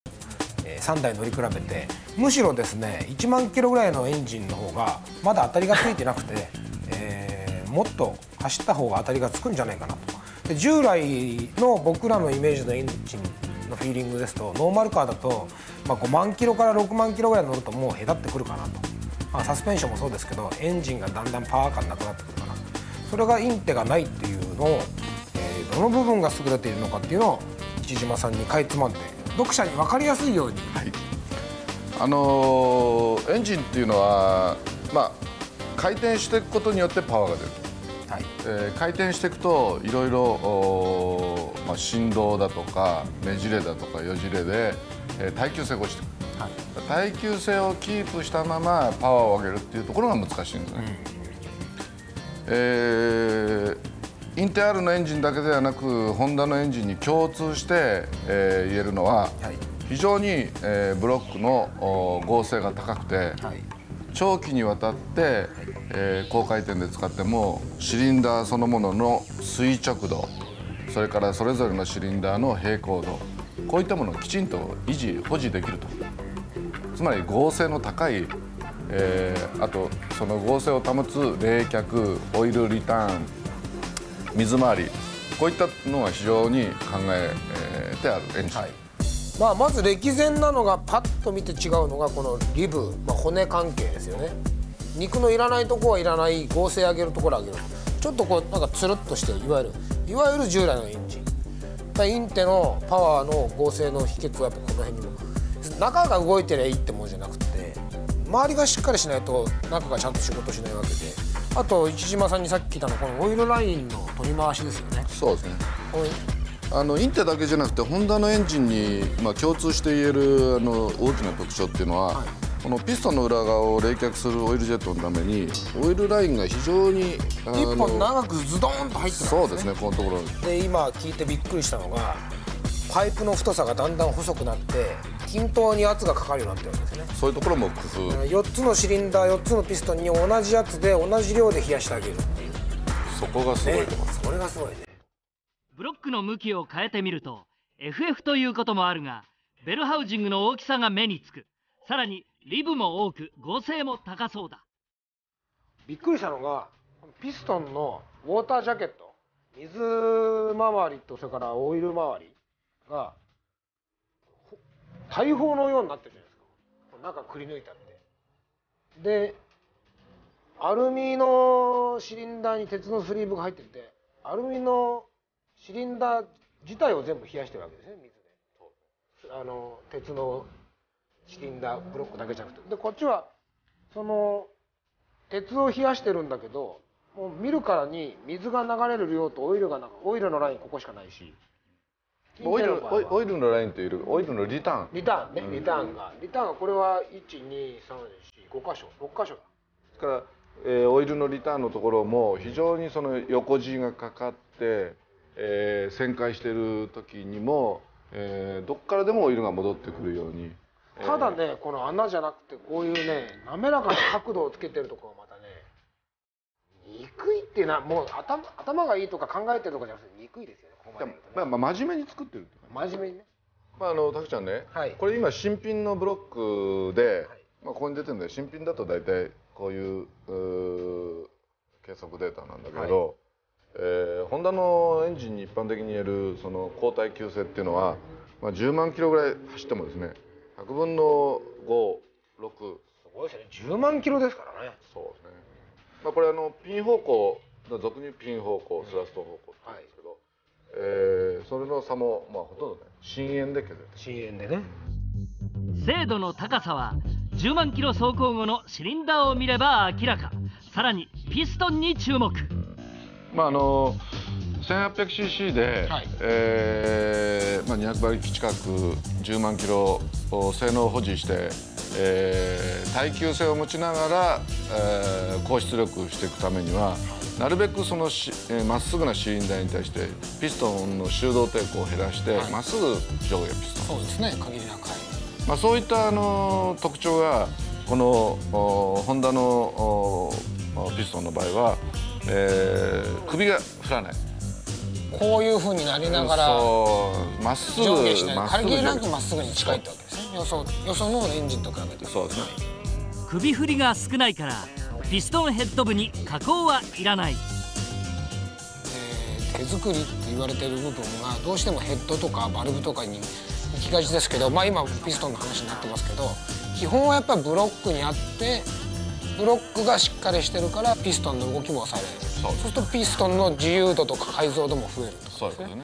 [ENG CC] B18C Engine in depth view with Spoon CEO 2001
Hondash - the ultimate tool to monitor your Honda
B18C Engine in depth view with Spoon CEO Ichishima Tatsuru